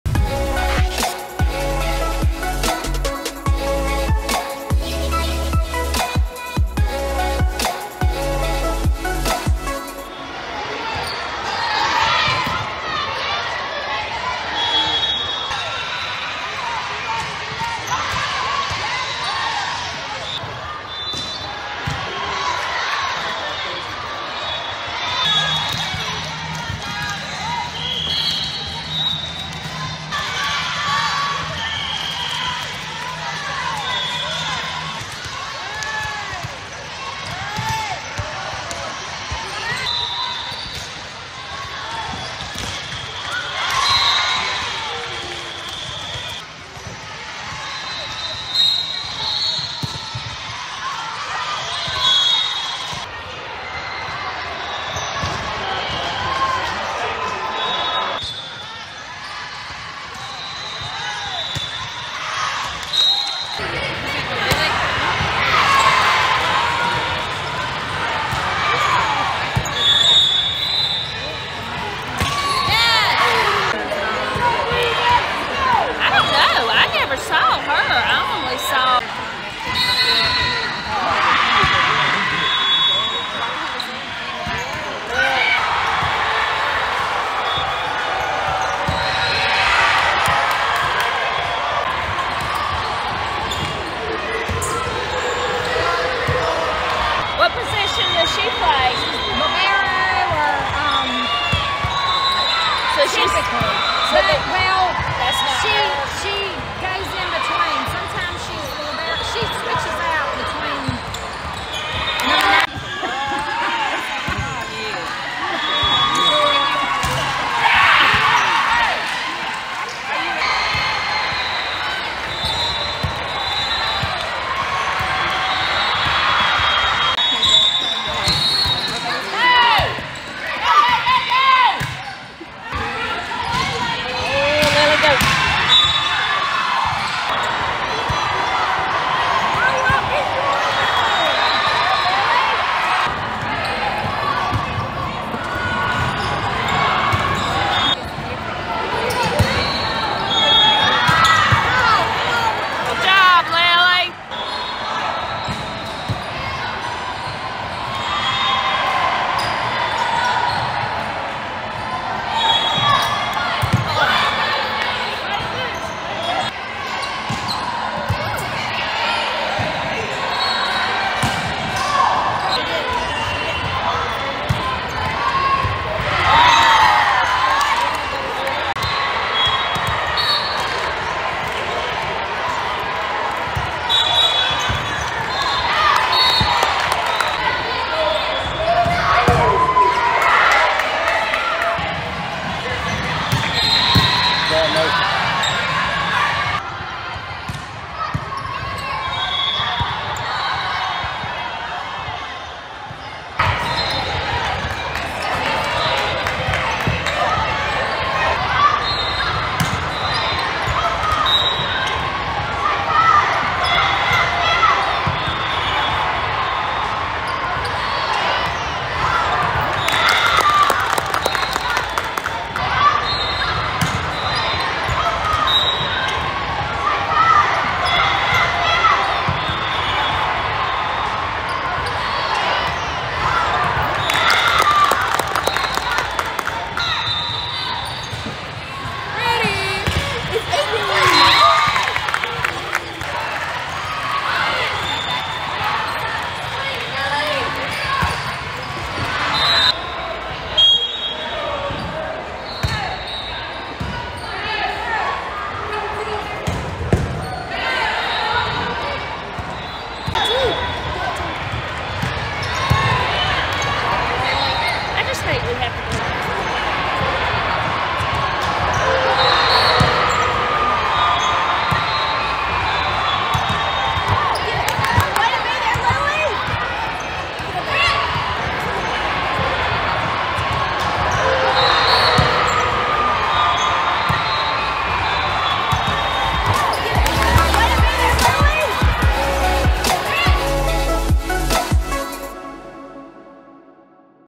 Lily Rudd 2023 volleyball highlights -- ScoutU
Lily Rudd is a 2024 L/DS/OH from Cambridge Academy in South Carolina.